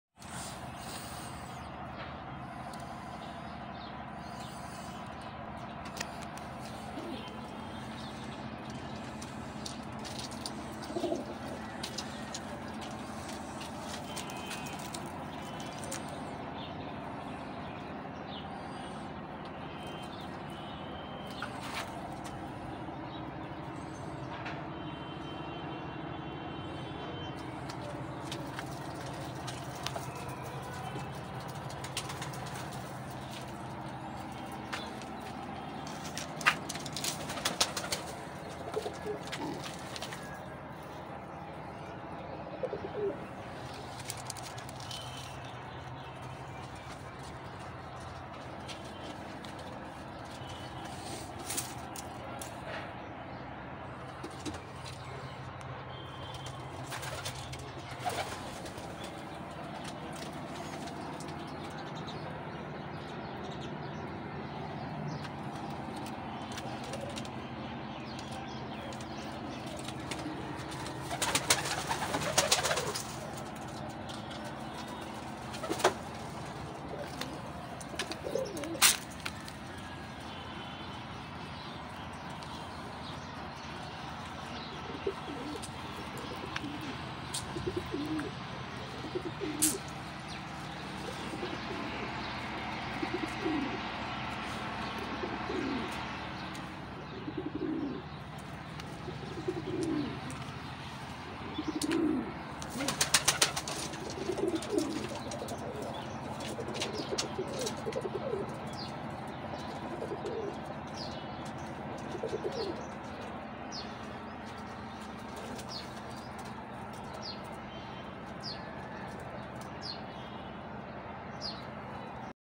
Pigeons cage Many fancy pigeons ❤😘❤😘 کبوتر کے پنجرے میں کبوتر کے انڈے اور بچے دے بوہت اقسم کے کبوتر
الحمام من أنواع كثيرة يعطي بيضه وأطفاله في قفص الحمام.. ❤😘❤😘 कई प्रकार के कबूतर कबूतर के अंडे और कबूतर के पिंजरे में बच्चे देते हैं shortvideo youtube trendingshorts birds wildlife pigeons fancypigeon @zarawild333